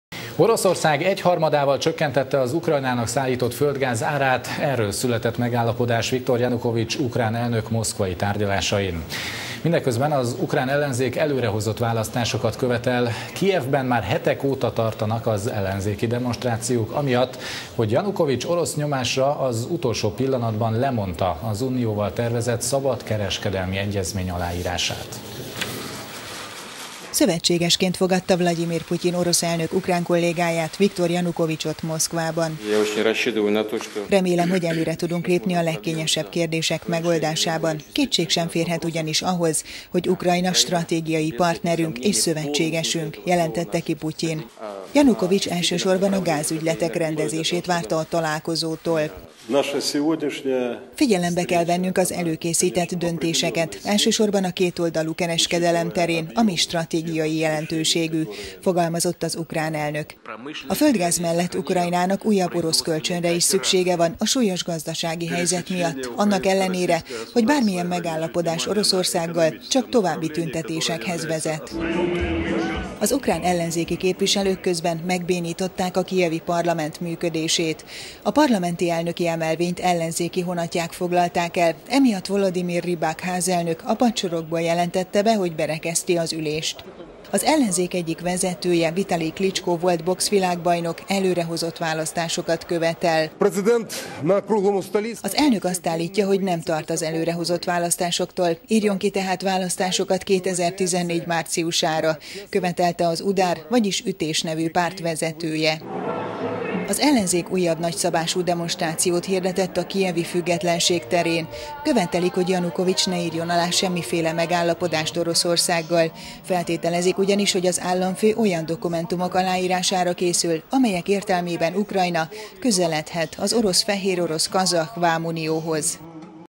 Klicsko ringbe hívta Janukovics elnököt
"Ringbe hívta" küzdelemre Vitalij Klicsko, az Ütés (UDAR) ökölvívóból lett pártvezetője Viktor Janukovics ukrán elnököt kedden a kijevi Függetlenség terén összehívott EU-párti nagygyűlésen.